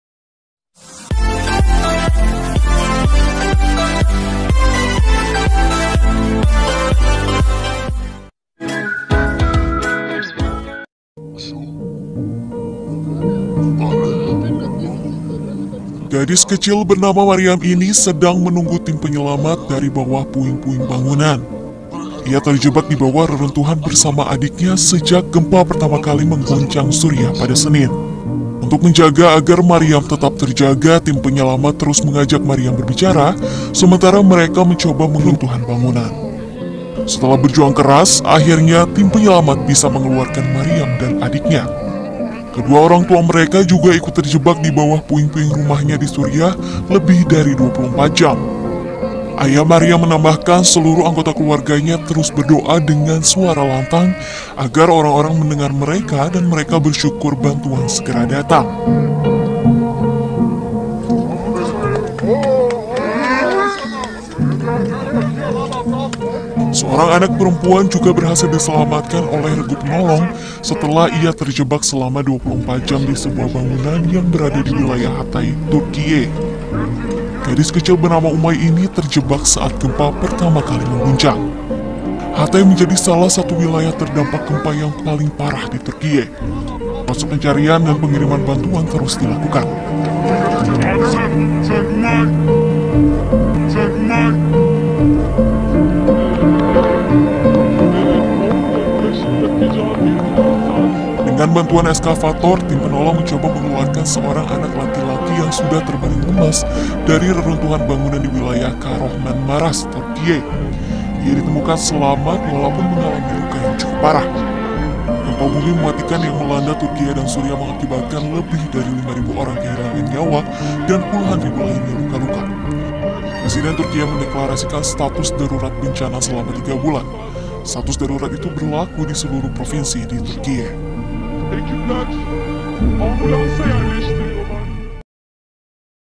tragedi gempa turki 2023
tragedidi yang memilukan  gempa turki yang sangat banyak memakan korban inilah salah satu kisah yang sangat memilukan tersebut